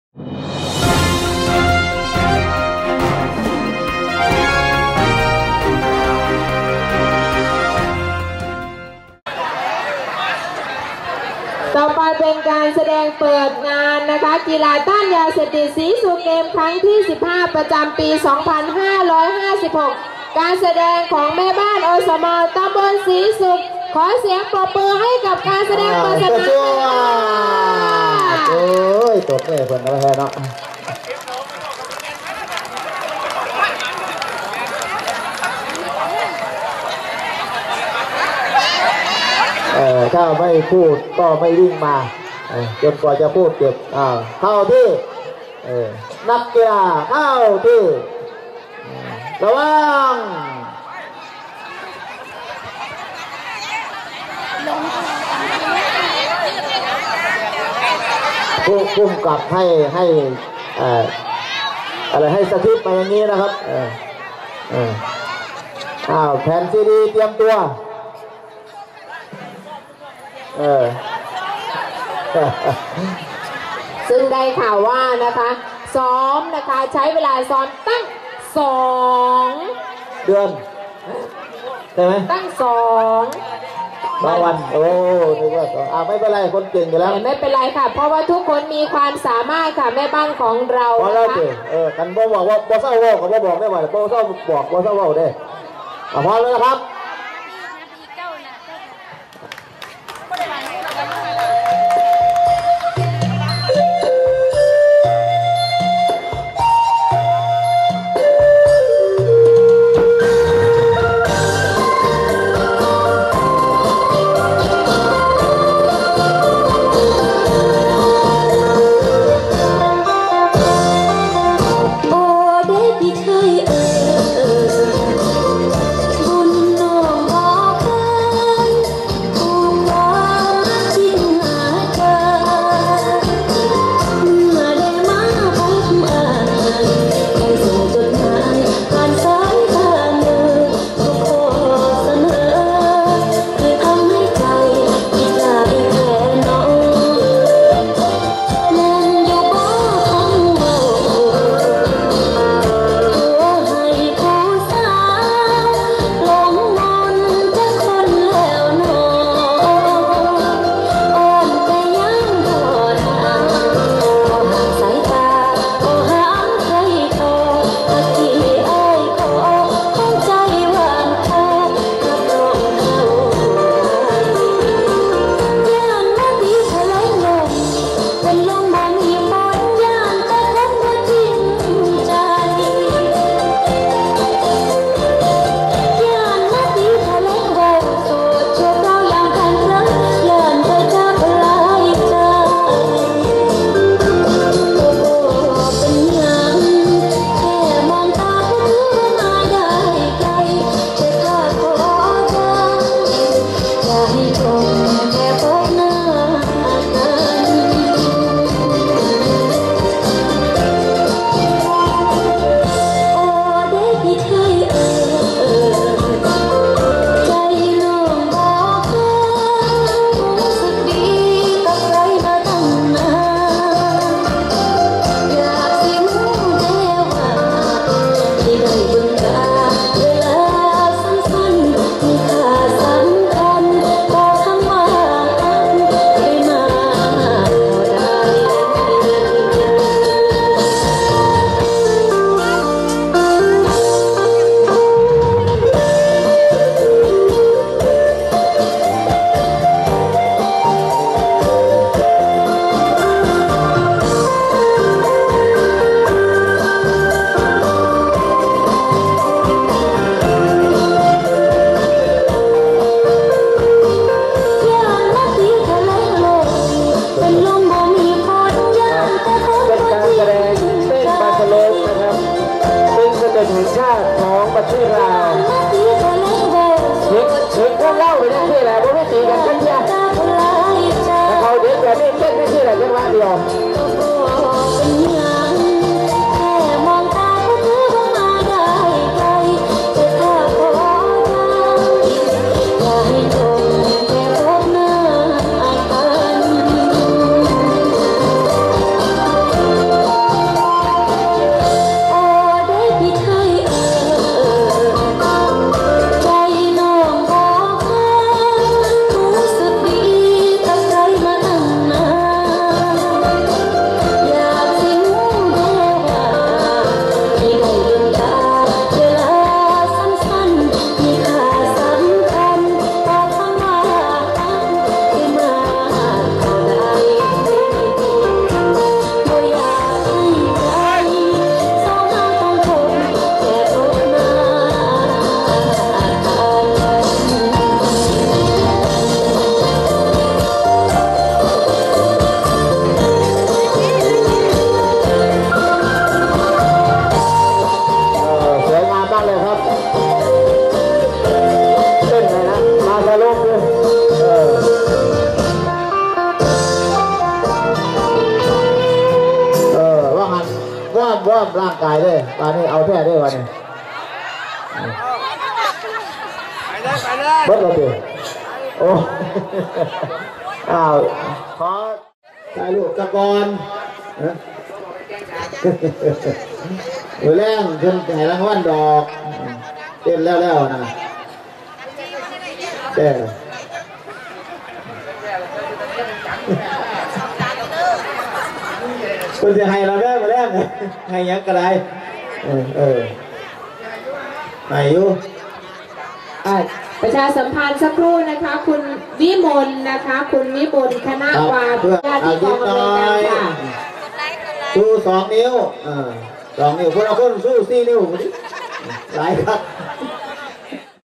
กลุ่มแม่บ้านและ อสม ตำบลศรีสุข แสดงในพิธีเปิด ศรีสุขเกมส์ 2556
ดร.พงษ์ศักดิ์ ตั้งวานิชกพงษ์ นายกองค์การบริหารส่วนจังหวัดขอนแก่น เป็นประธานในพิธีเปิด การแข่งขันกีฬาต้านยาเสพติด ประจำปีงบประมาณ 2556 " ศรีสุขเกมส์ เกมส์ ครั้งที่ 35 " ระหว่างวันที่ 25-27 ธันวาคม 2556 ณ สนามโรงเรียนบ้านศรีสุข ตำบลศรีสุข อำเภอสีชมพู จังหวัดขอนแก่น